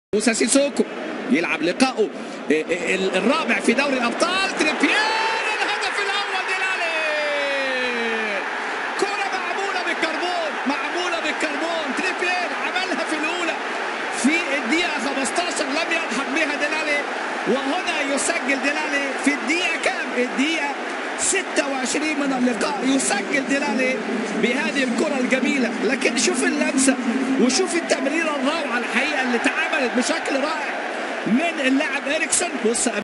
هدف ديلي الي الاول في ريال مدريد !!!! هدف تسللل
هدف ديلي الاول الاول علي ريال مدريد !!!